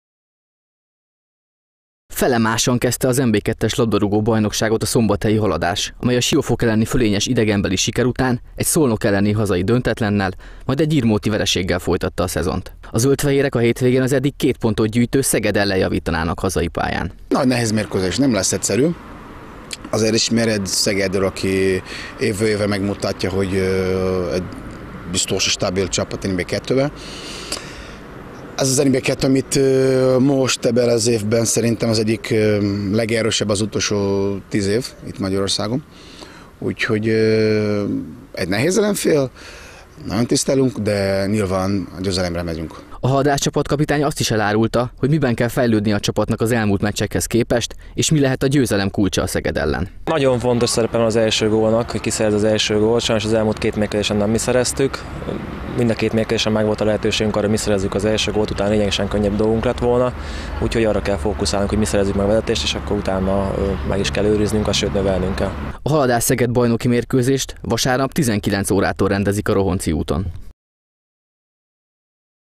Hazai pályán javítana a Szombathelyi Haladás
A zöld-fehérek a hétvégén a Szeged együttesét fogadják az NB II-es labdarúgó bajnokság 4. fordulójában.